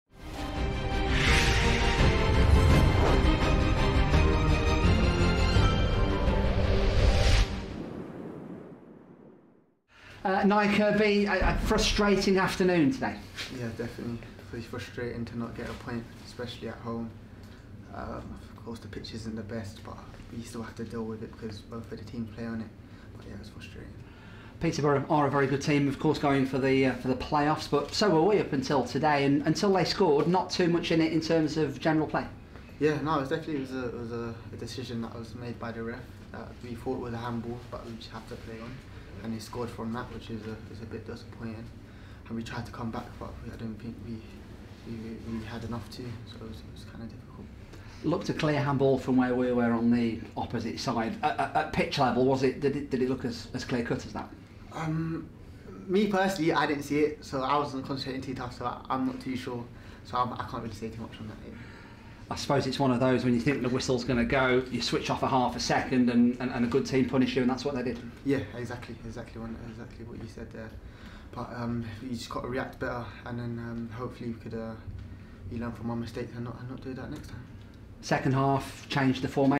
Peterborough Reaction | Nya Kirby
Nya Kirby speaks to the media after making his second consecutive start in defeat to Peterborough.